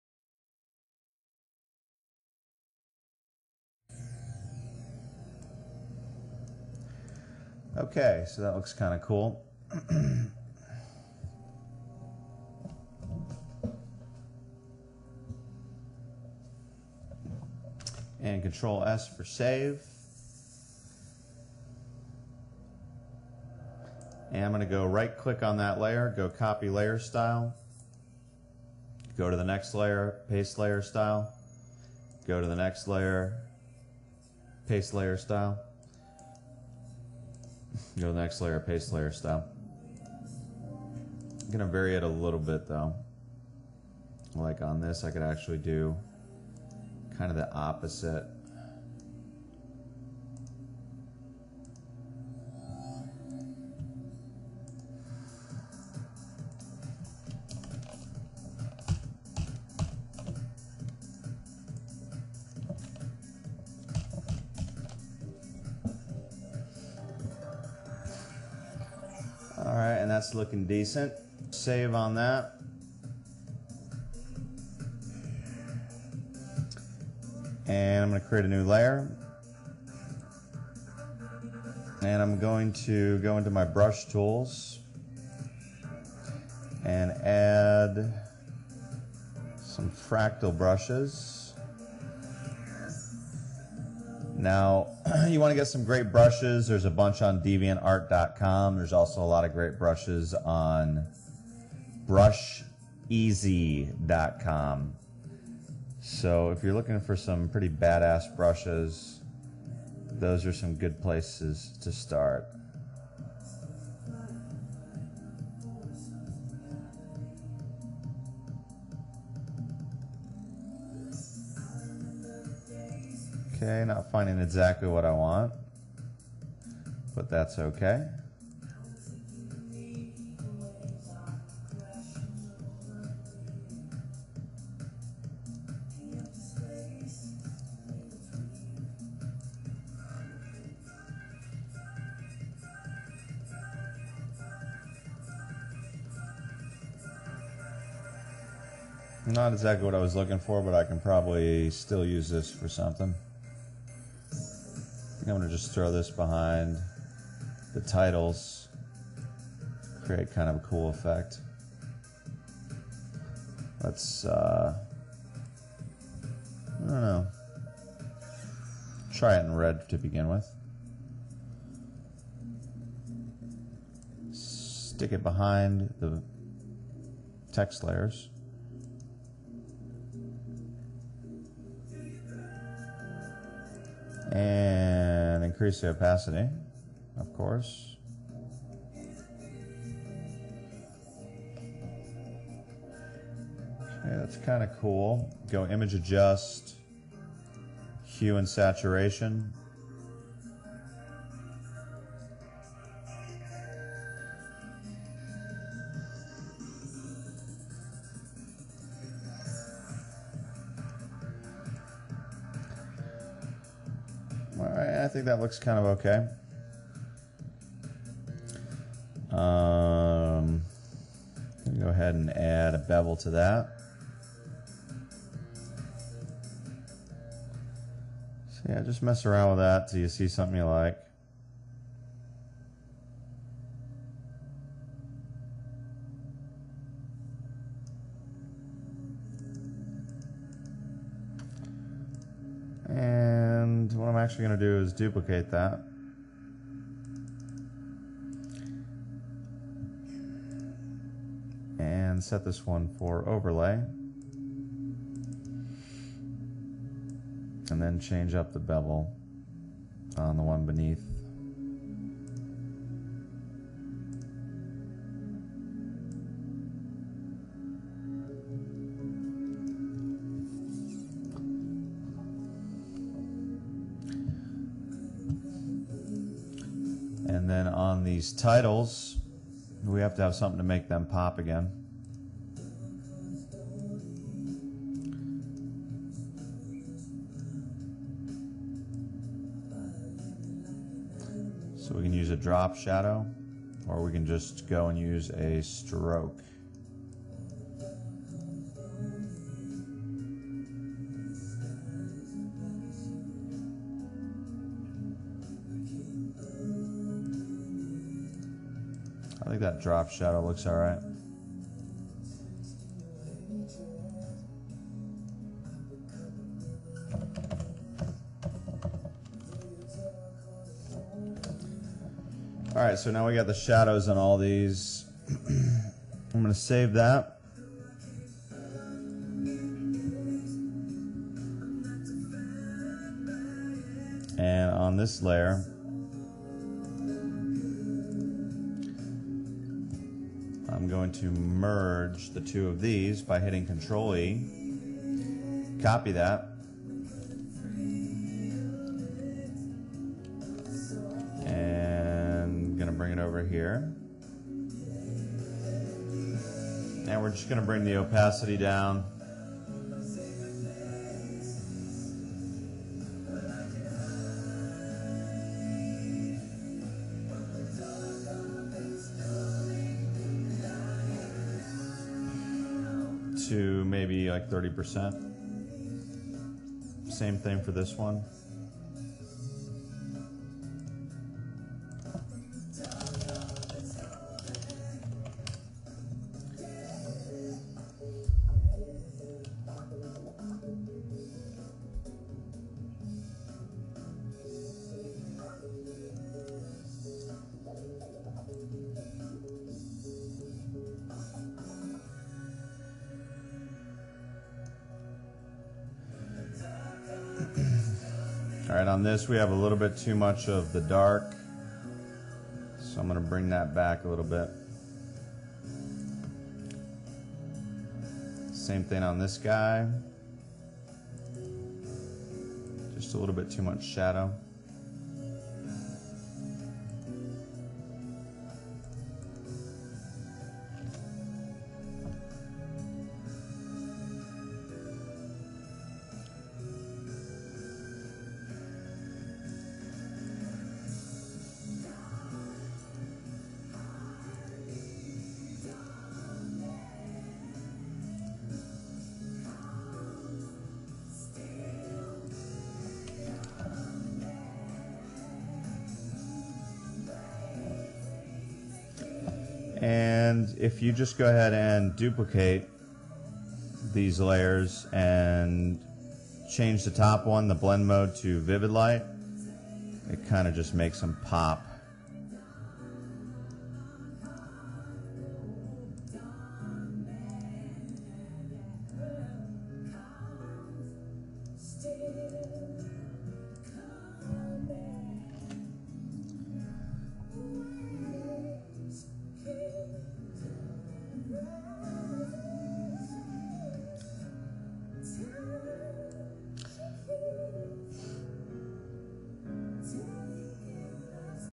Photoshop Club Flyer Tutorial X: Layer Styles Custom Brushes
This is a Photoshop tutorial for designing a club flyer from 2008! Why are you watching this?

This is a multi-part tutorial from Wikkid Things that shows you how to make a club flyer design in Photoshop. Part X teaches you how to work with layer styles & custom brushes.